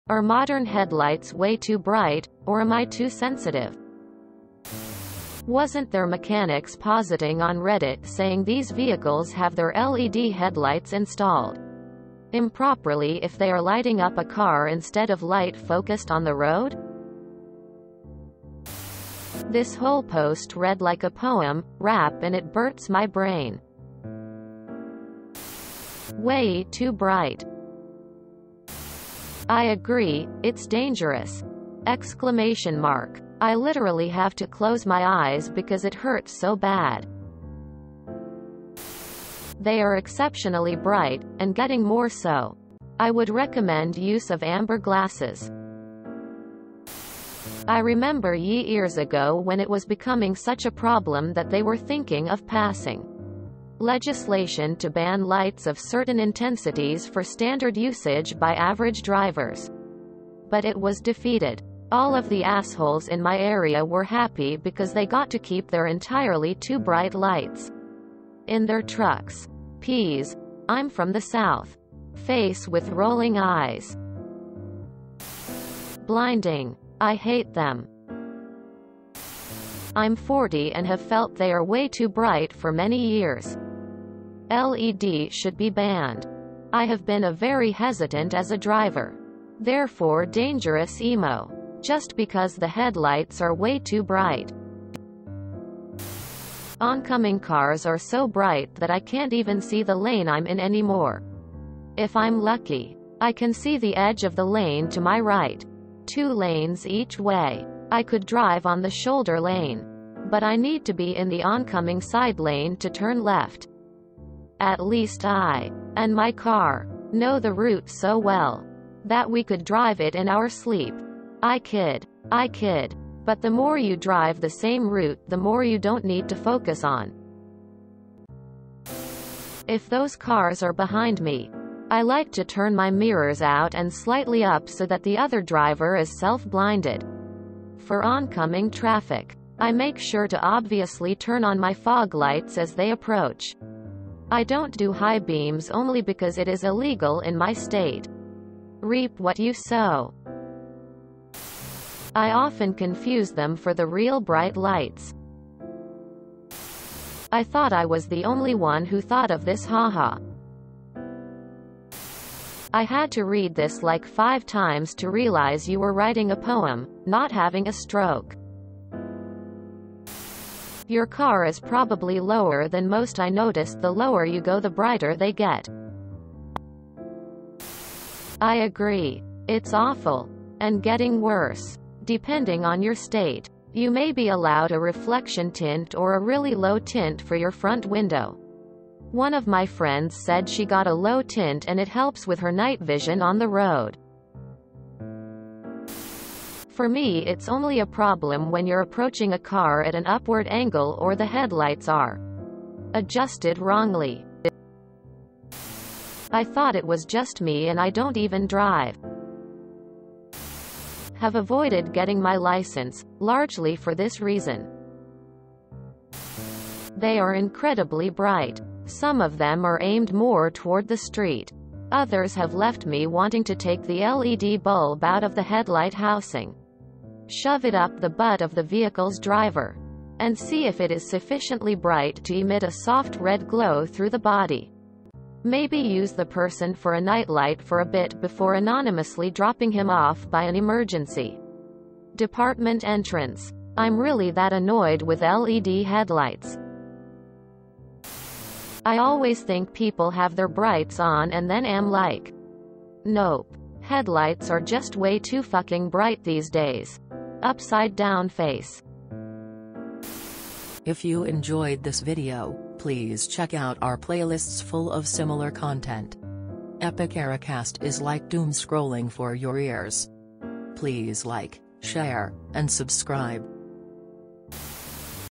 Are modern headlights WAY TOO bright, or am I too sensitive?
Are modern headlights WAY TOO bright, or am I too sensitive? (r/TooAfraidToAsk)

Source: owlbehome on Reddit (link removed because YouTube keeps dinging me for linking to a "site with nudity"!?)